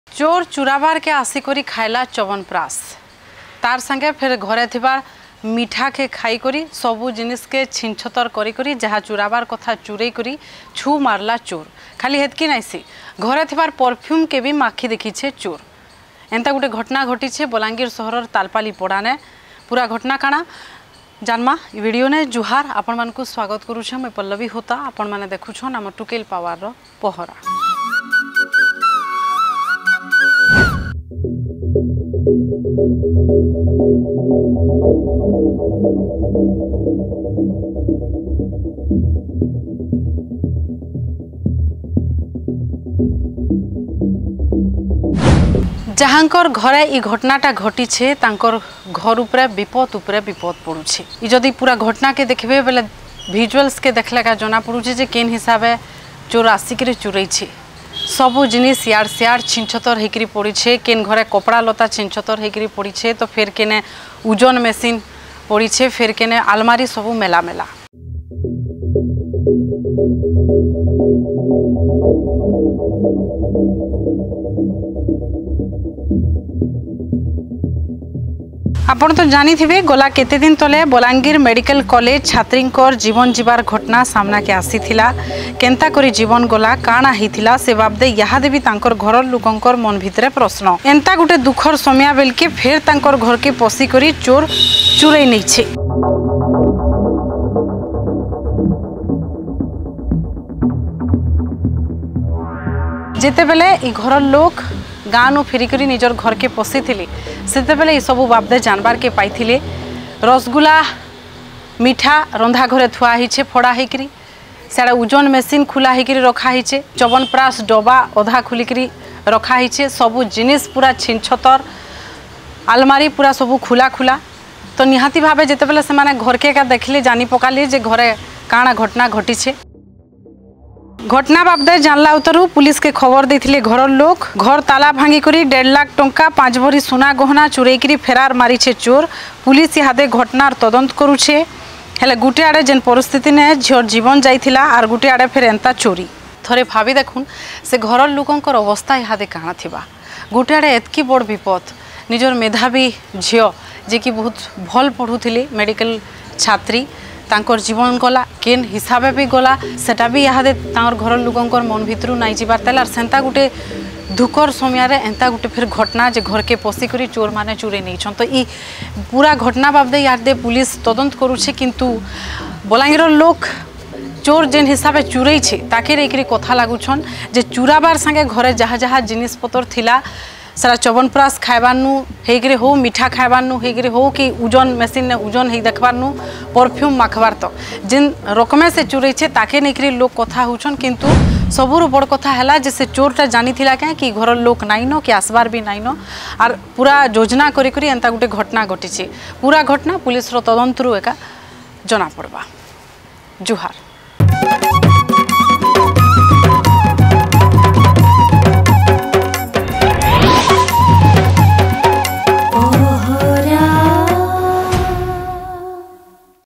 ଚୁରାବାରକେ ଆସି ମିଠା, ଚବନପ୍ରାସ୍ ଖାଏଲା, ପରଫ୍ୟୁମ୍ ବି ମାଖଲା ଚୋର...
ଚୋର ଚୁରାବାରକେ ଆସିକରି ଖାଏଲା ଚବନପ୍ରାସ୍ । ତାର ସାଙ୍ଗେ ଘରେ ଥିବାର୍ ମିଠାକେ ଖାଇକରି ଉଜନ୍ ବି ହେଇକରି ସବୁ ଜିନିଷ୍ ପତର ଛନି ଛତର କରି ଯାହା ଚୁରାବାର କଥା ଚୁରେଇକରି ଛୁ । ଖାଲି ଏତକି ନାଇଁସେ ଘରେ ଥିବାର୍ ପରଫ୍ୟୁମ୍ ବି ମାଖିଦେଖିଛେ ଚୋର ।  ଏନ୍ତା ଗୁଟେ ଘଟନା ଘଟିଛେ ବଲାଙ୍ଗିର ସହରର ତାଲପାଲି ପଡାନେ ।